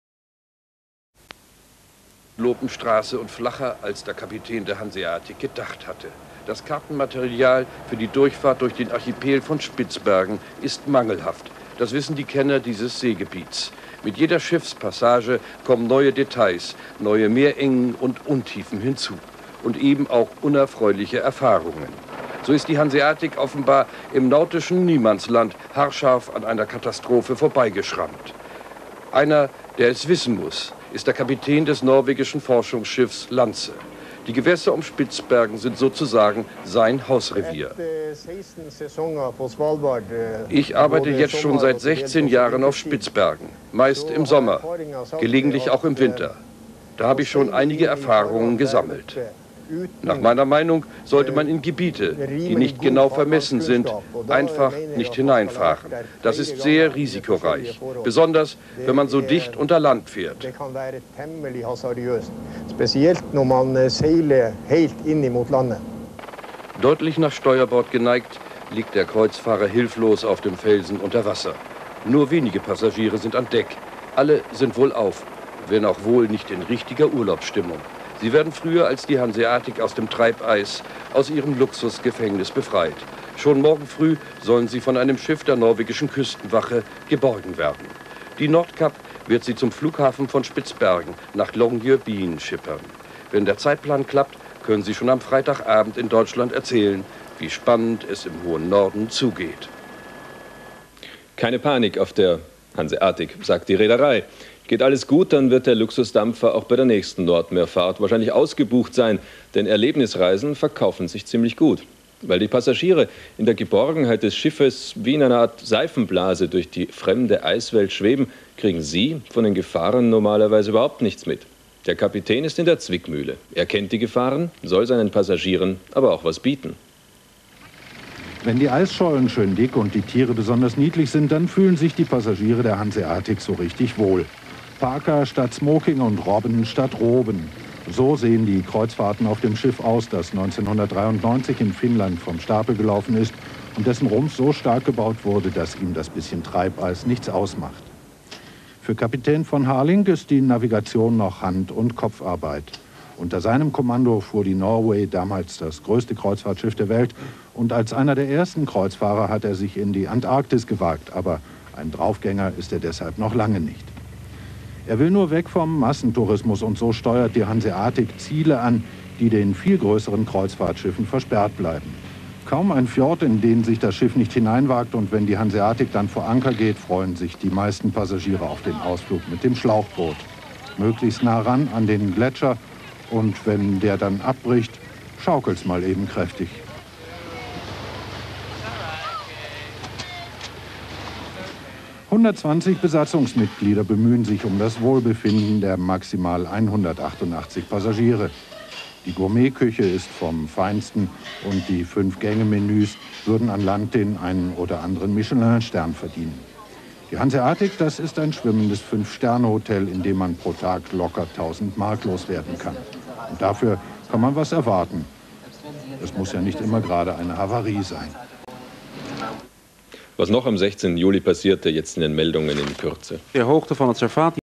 Schiffbruch Hanseatic Spitzbergen Nordpol 1997
Die Hanseatic is gerade mal nicht weiter gekommen im schweren Titanic Eis.